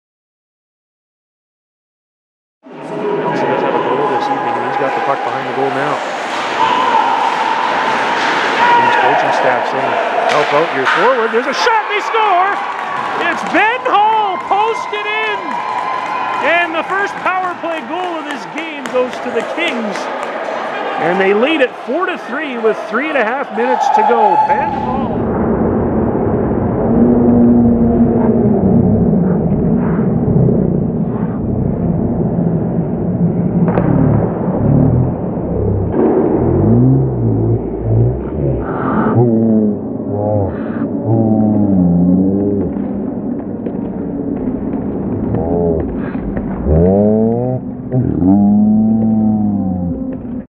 Kings Fourth Goal @ Winnipeg (Feb 12)
Ben Hull goes post and in on the PP